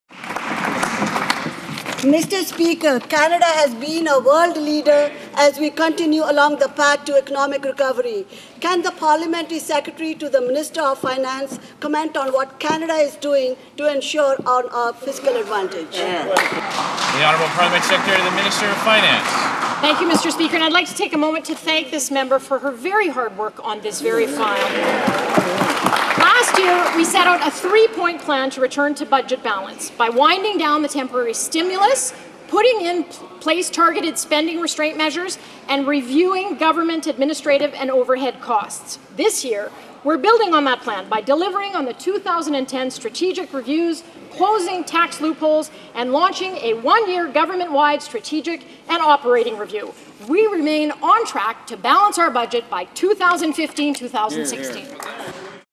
Canada's Economic Recovery
Mr. Speaker, Canada has been a world leader as we continue along the path to economic recovery. Can the Parliamentary Secretary to the Minister of Finance comment on what Canada is doing to ensure our fiscal advantage?

Shelly Glover, Parliamentary Secretary to the Minister of Finance:Mr. Speaker, I would like to take a moment to thank this member for her very hard work on this very file.

Last year we set out a three-point plan to return the budget to balance by winding down the temporary stimulus, putting in place targeted spending restraint measures, and reviewing government administrative and overhead costs.

This year we are building on that plan by delivering on the 2010 strategic reviews, closing tax loopholes and launching a one-year government-wide strategic and operating review.